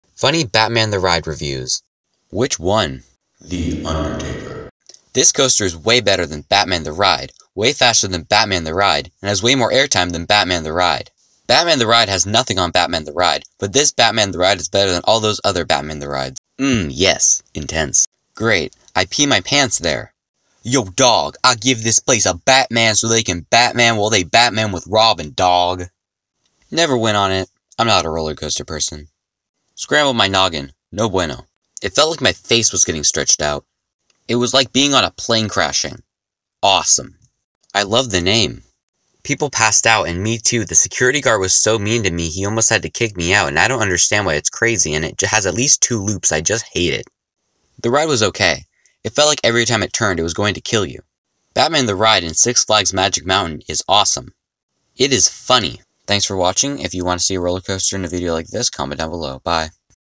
Funny Batman: The Ride Google reviews
Inspired by Coaster Journey's series of the same name.